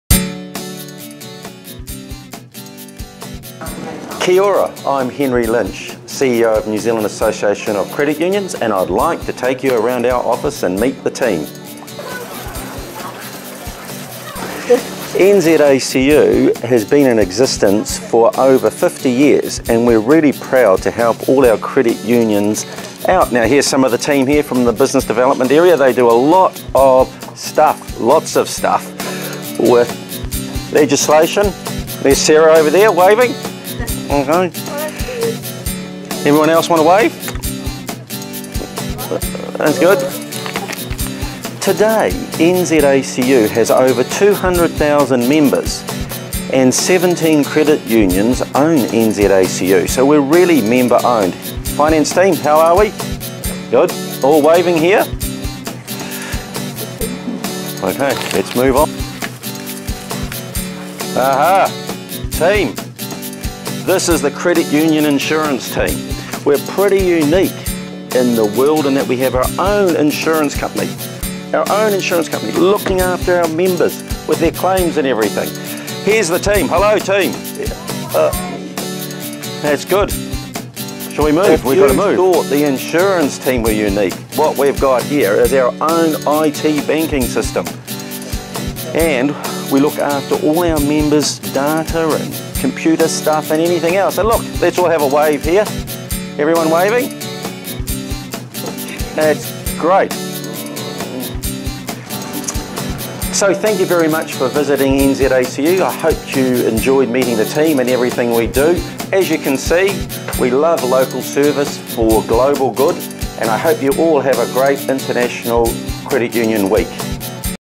Tour of New Zealand Association of Credit Unions
Henry Lynch, CEO of the New Zealand Association of Credit Unions, takes CUToday.info on a tour of his association's offices as part of CUToday.info's salute to International Credit Union Week 2014.